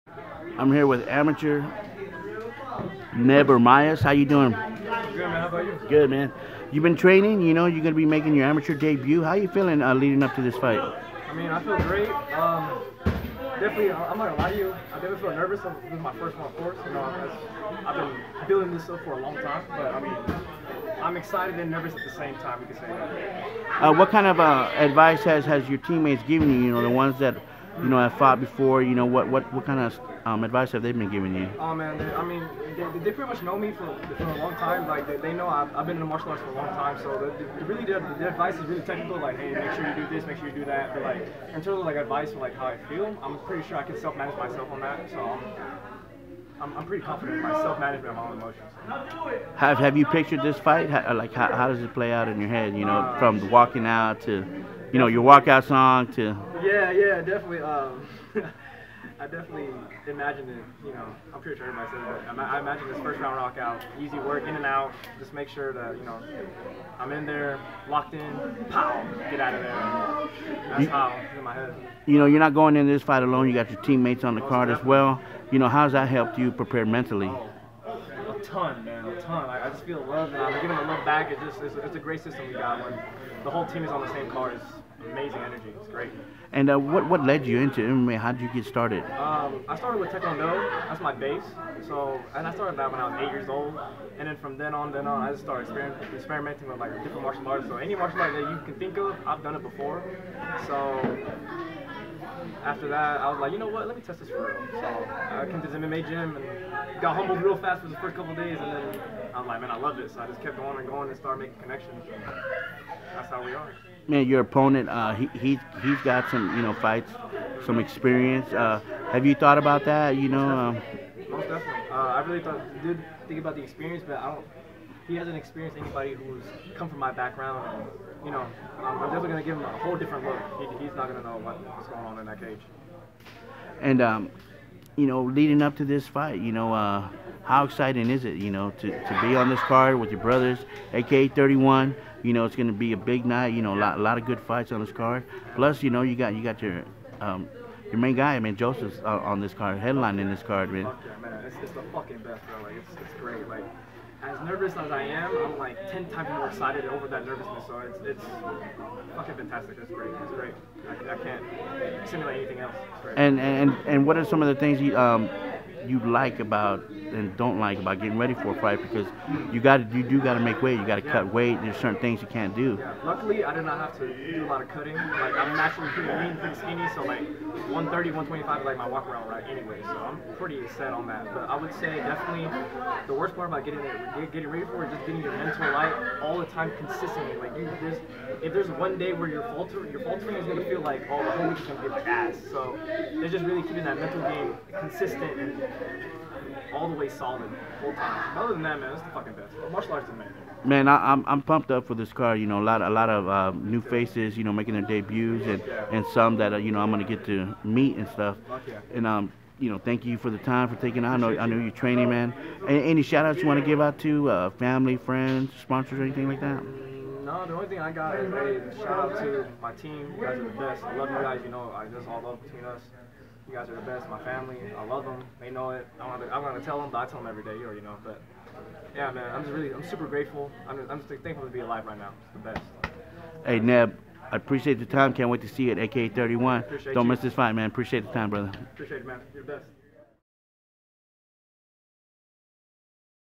NEB ERMIAS PRE-FIGHT INTERVIEW
@akafights 31
Saturday August 26, 2023
Marksville, Louisiana

Neb Ermias vs Shon Brown